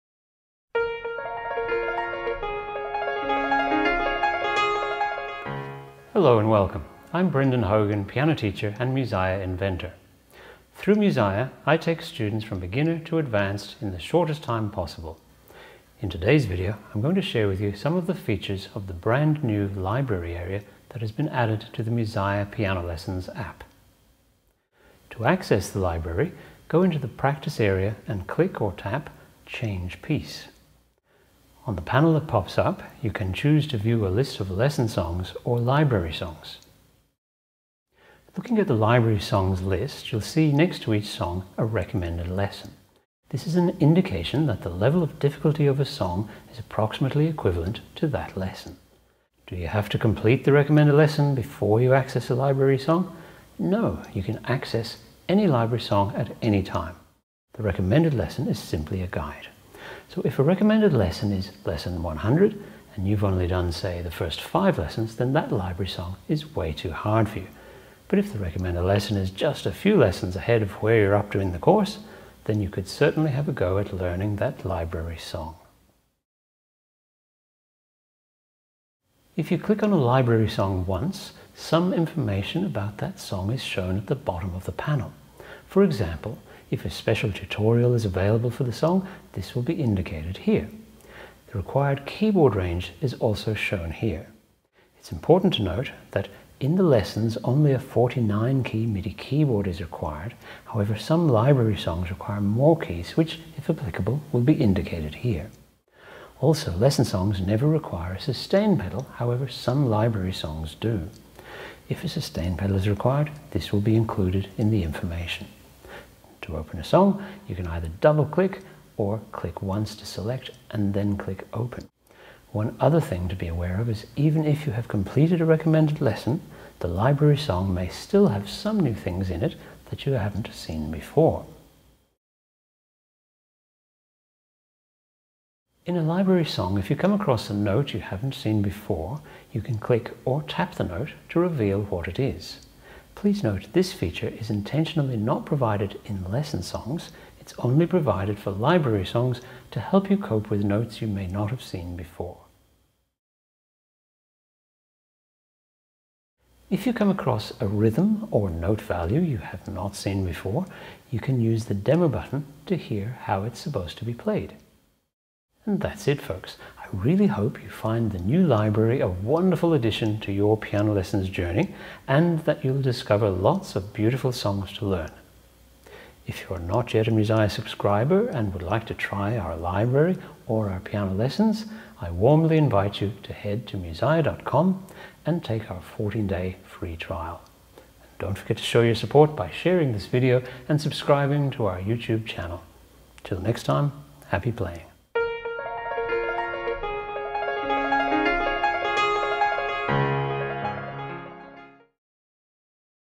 Piano Song Library | Musiah
Introduction to Musiah's Piano Song Library.

Discover the main features of our piano library which offers students the opportunity to optionally learn extra songs in the Musiah app's Practice Area as supplementary pieces to the songs they learn in their lessons.

Songs in the library are non copyright material from a variety of genres and are sorted by level so students can easily discern what lessons they need to have completed in order to be able to play a particular song.